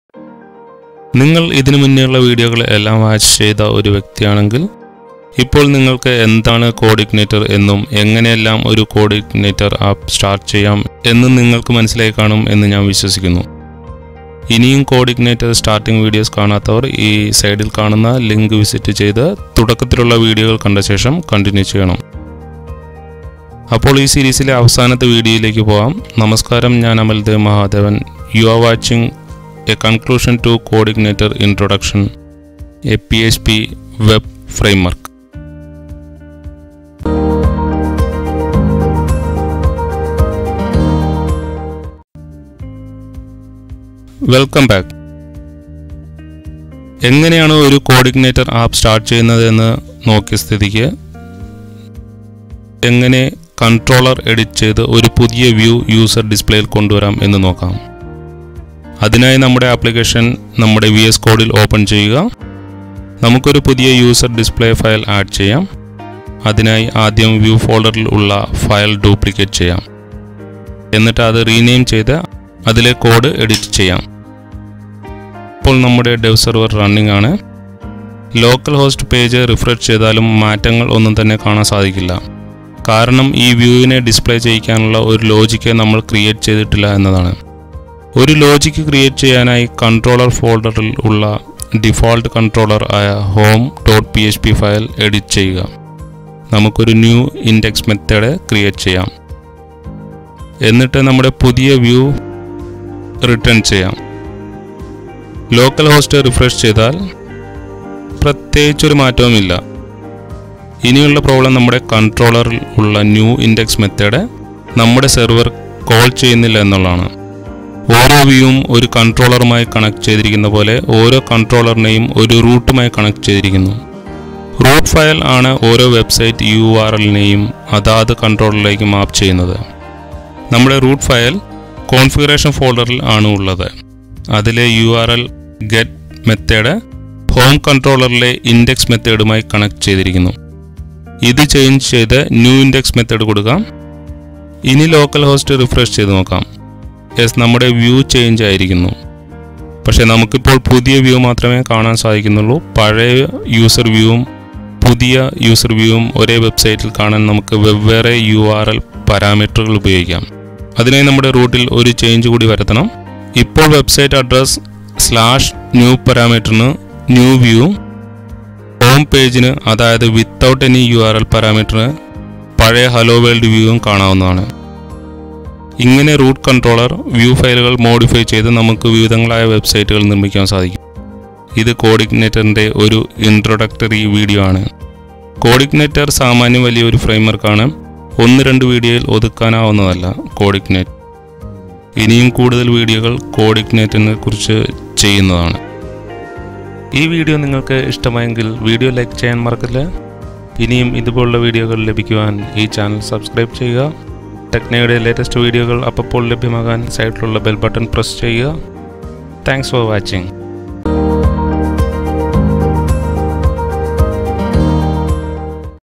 CodeIgniter in Malayalam - A PHP Web framework | Part-6 A conclusion to CodeIgniter introduction
I made this video as conclusion to CodeIgniter web framework. CodeIgniter very simple, flexible and powerful framework from PHP world. Learning CodeIgniter is very simple.